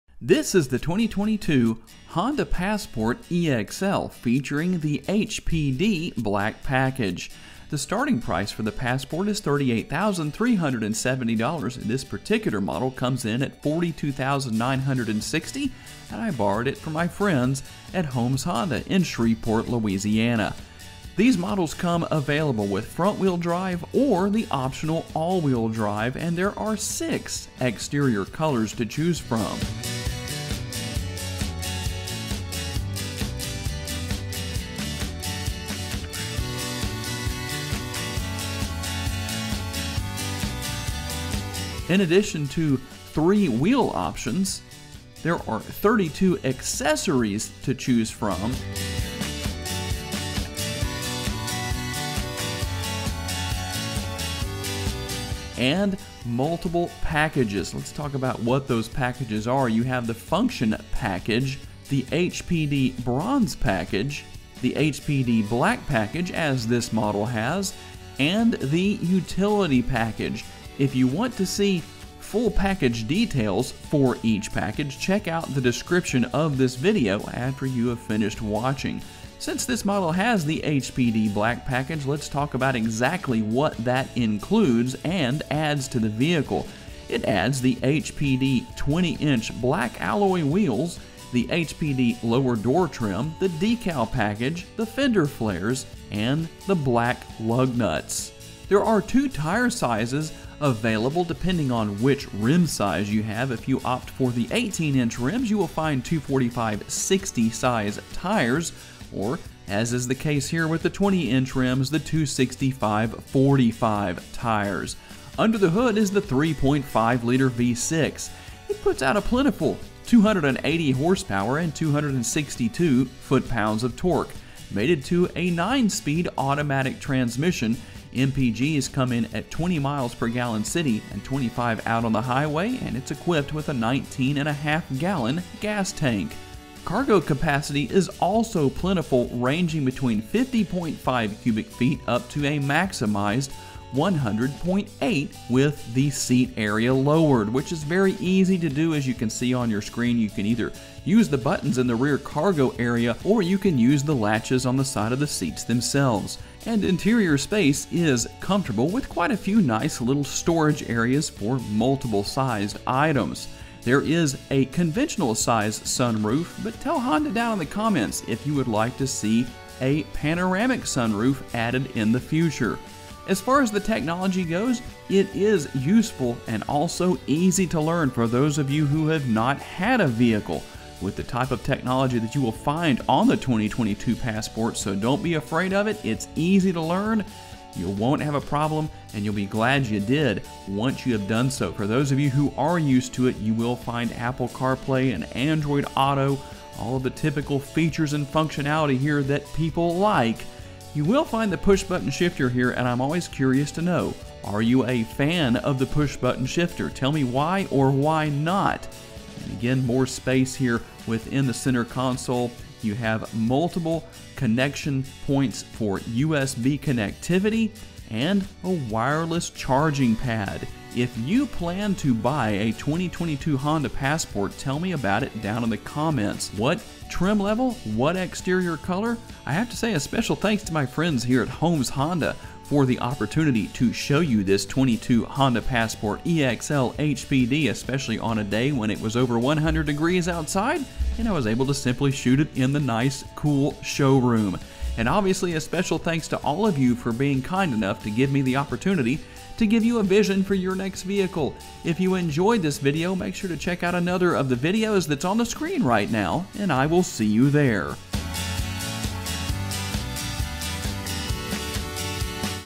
2022 Honda Passport EX-L HPD - The Passport With Something Extra
The Honda Passport has long been a popular two-row mid-size SUV among buyers. While the sportiness is there, when adding a multitude of features to customize your Passport, one of the more obvious choice is the HPD (Honda Performance Development) package. See this package on a 2022 Passport EX-L in today’s Vehicle Visionary video!

2022 Honda Passport Package Details

The Function Package includes the following:
Cargo Net
Cargo Cover
First-Aid Kit

The HPD Bronze Package includes the following:
HPD™ 20-Inch Bronze Alloy Wheels
HPD™ Lower Door Trim
HPD™ Decal Package
Fender Flares

The HPD Black Package includes the following:
HPD™ 20-Inch Black Alloy Wheels
HPD™ Decal Package
Fender Flares
Black Wheel Lug Nuts

The Utility Package includes the following:
Crossbars
Roof Rails, Gloss Black
Trailer Hitch